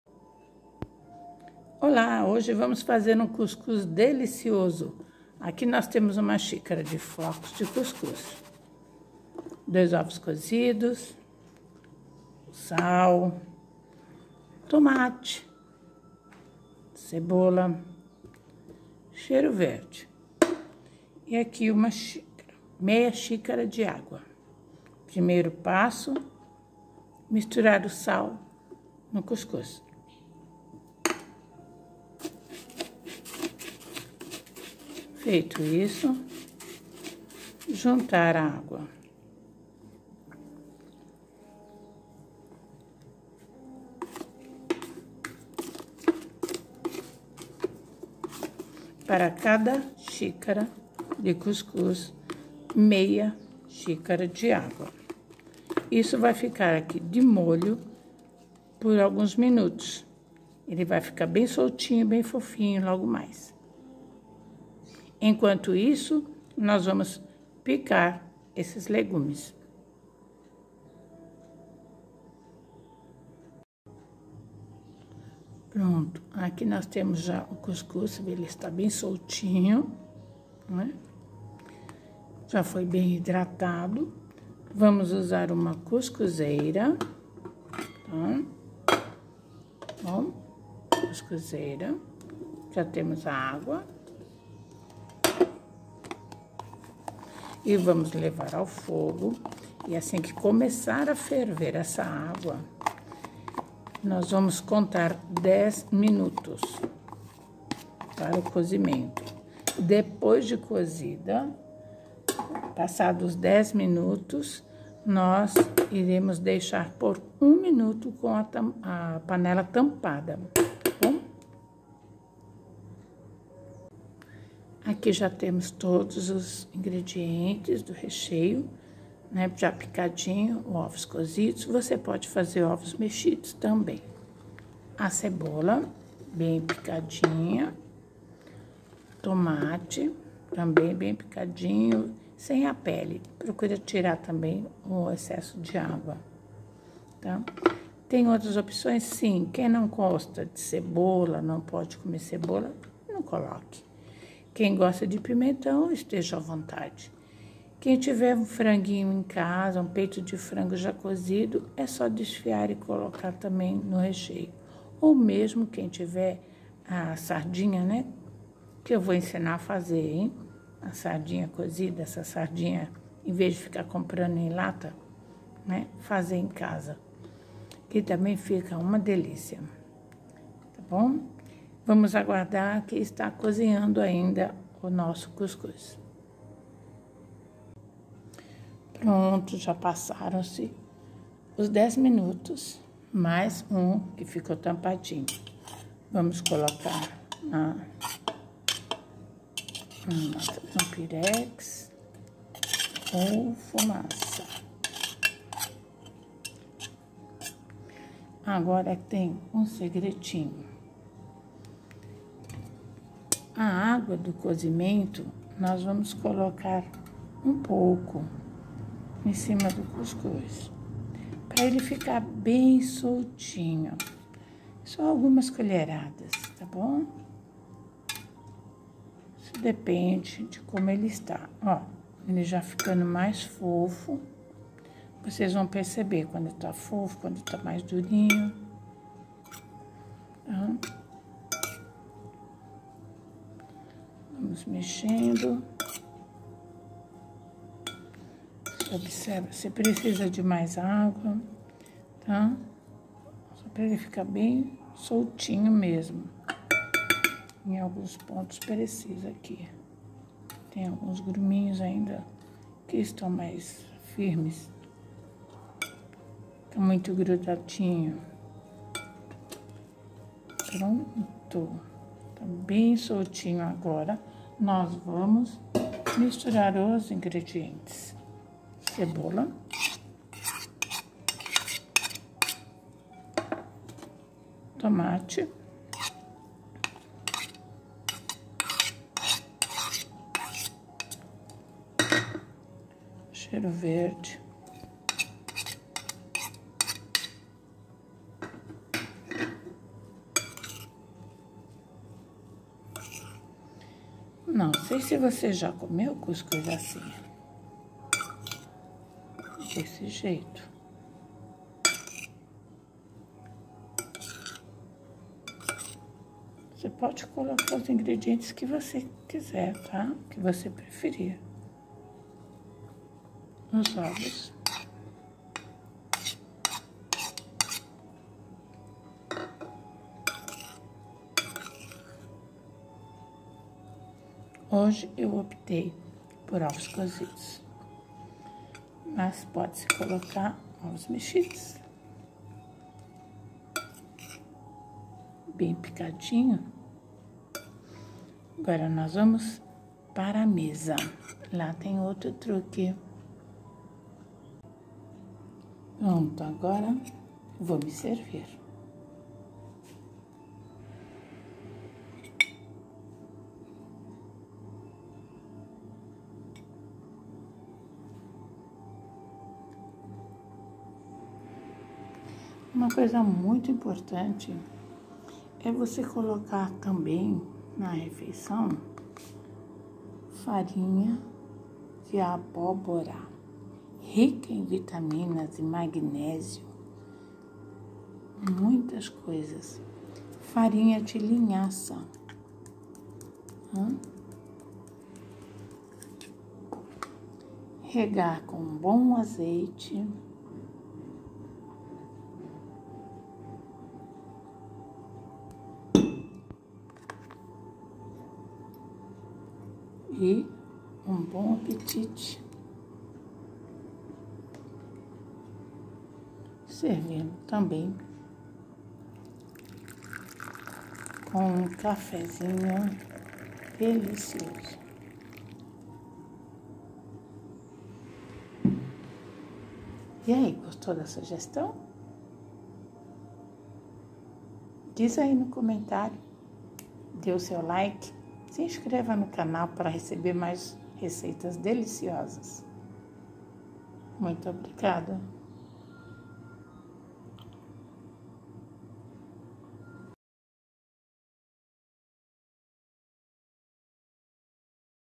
Você também faz seu cuscuz assim?
Esse cuscuz fica muito gostoso e  nutritivo. Faço no café da manhã, e as vezes no jantar aí eu coloco frango desfiado para enriquecer mais o prato.
Ingredientes: 1 xícara de cuscuz flocos, e colher de chá de sal e 1/2 xícara de água para  hidratar, deixar descansando  por alguns minutos. colocar na cuscuzeira assim que começar a ferver a água conte 10 minutos, apague o fogo e deixe por 1 minuto. Use um pouco da água quente do cozimento para umedecer o cuscuz para ficar bem soltinho.
A parte pique 1 cebola, 1 tomate, cheiro verde, ovos cozidos ou mexidos, (pode colocar pimentão picado, frango desfiado, ou mesmo sardinha em lata, ou atum).
Coloco farinha de semente de abóbora e farinha de linhaça. Pois são muito importantes para o nosso organismo.